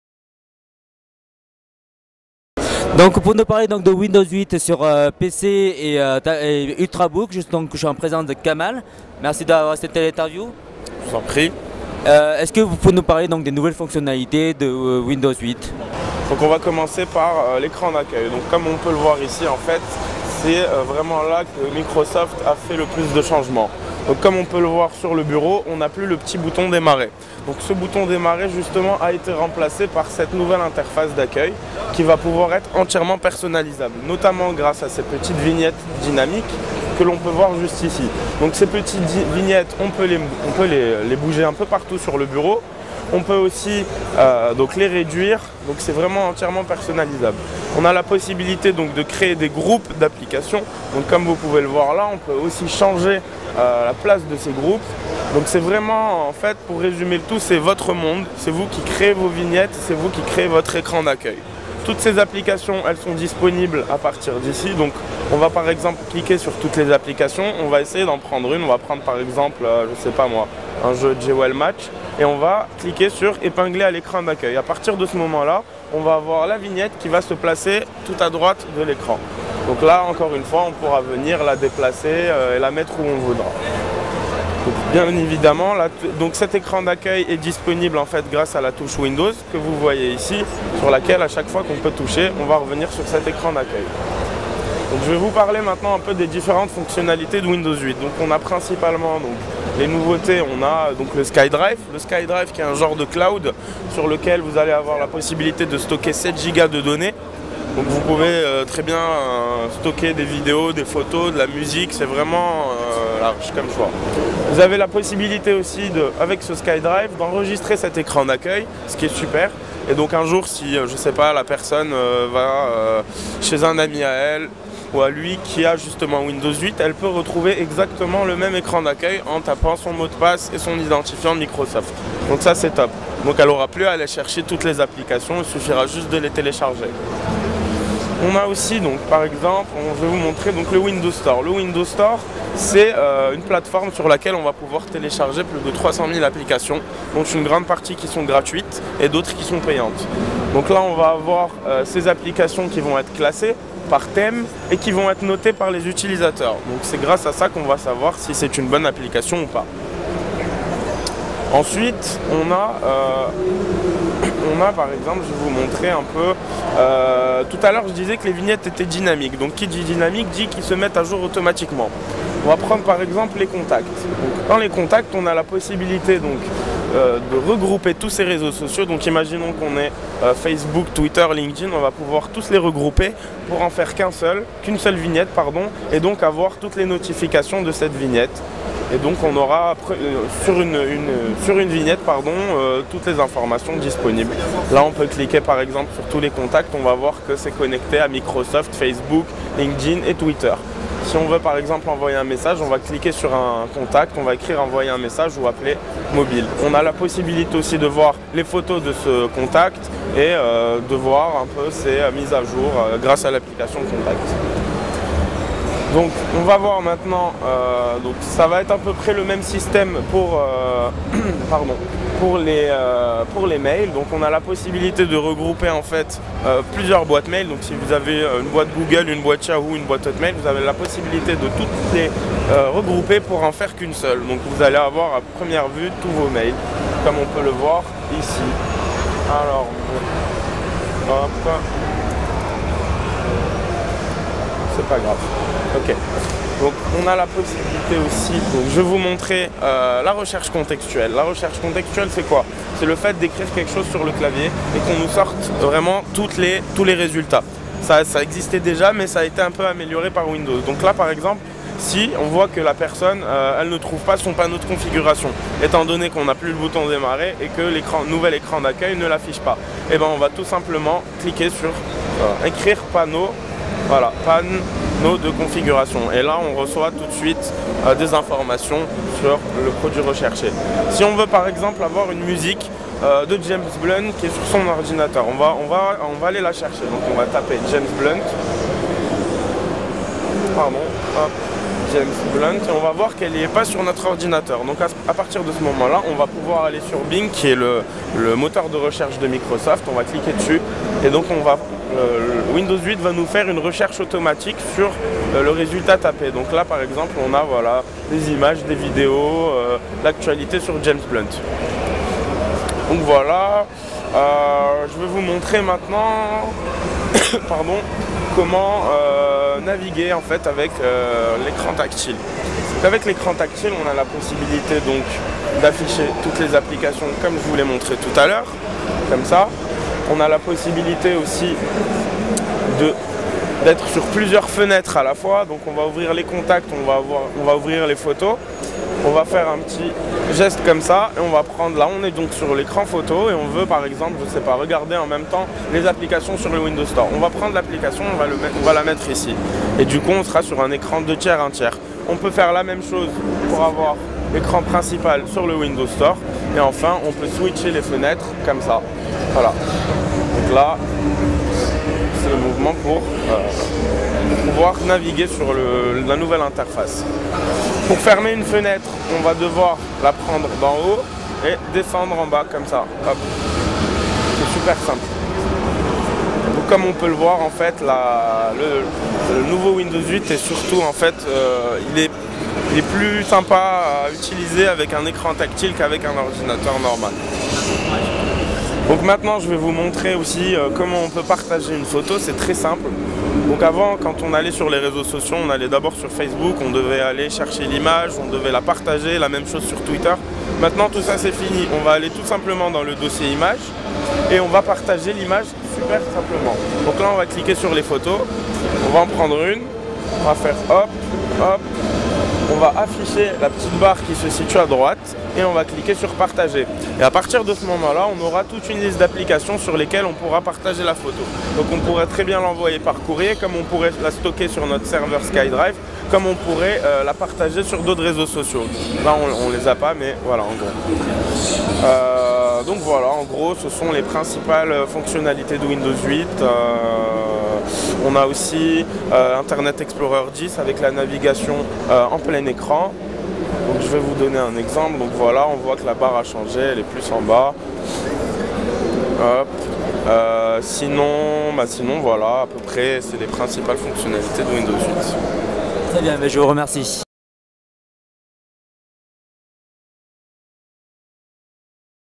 [PGW 2012] Windows 8 sur PC
Présentation sur le stand Microsoft du dernier système d'expoitation sur PC, Windows 8!